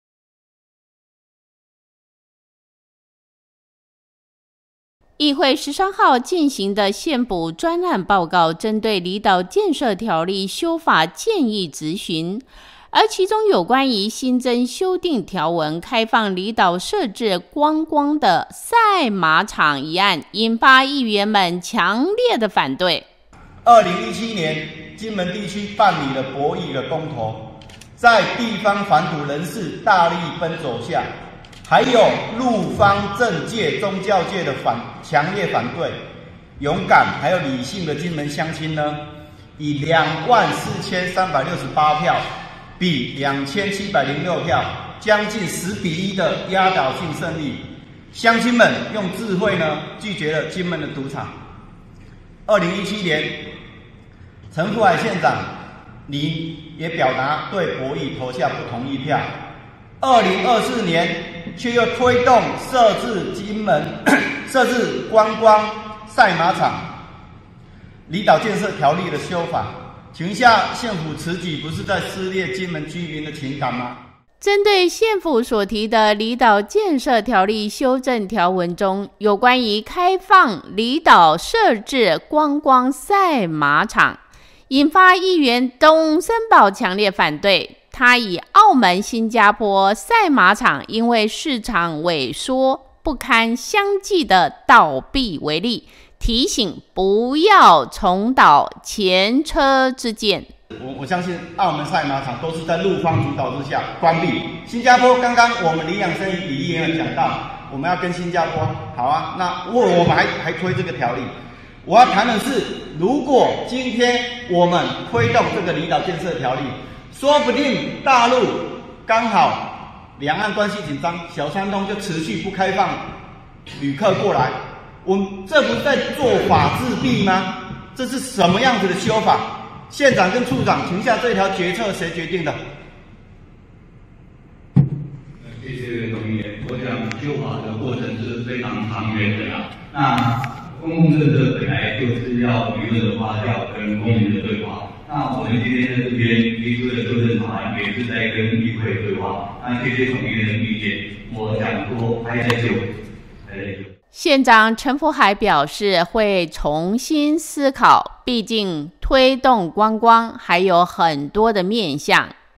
名城報報-金門縣府擬設觀光賽馬場 議員強烈反對
金門縣議會第八屆第三次定期會(13)日進行縣府專案報告，針對「離島建設條例」修法建議質詢，其中有關新增修正條文第10條之3「開放離島設置觀光賽馬場」乙案，引發議員董森堡強烈反對！他也以澳門、新加坡賽馬場因市場萎縮不堪虧損相繼倒閉為例，提醒勿蹈前車之鑑，加上目前兩岸關係持續緊張，萬一陸客從此不來，此舉無疑作繭自縛。

針對縣府所提「離島建設條例修正條文第10條之3：開放離島設置觀光賽馬場，觀光賽馬場之申請程序、設置標準、相關監督管理等事項實施辦法，由離島縣政府定之，特許經營觀光賽馬場及從事觀光賽馬者，不適用動物保護法相關規定及刑法賭博罪章之規定」乙案，議員董森堡表達堅決反對之意見，他表示，縣長陳福海過去擔任立委期間，與時任澎湖縣立委林炳坤，利用離島建設條例的漏洞，修法通過了10之2《開放離島設置觀光賭場》條例，後續澎、金、馬地區因促賭、反賭，辦理了多場公投，嚴重撕裂了島嶼民眾的情感。

對此，縣長陳福海表示要重新思考，畢竟要推動觀光還有很多的方向可以努力，對於牽涉到「賭場」這兩個字，他也表示堅決的反對；未來重大方案的推動一定要結合議員的聲音、府會共同討論，一起努力讓金門更好。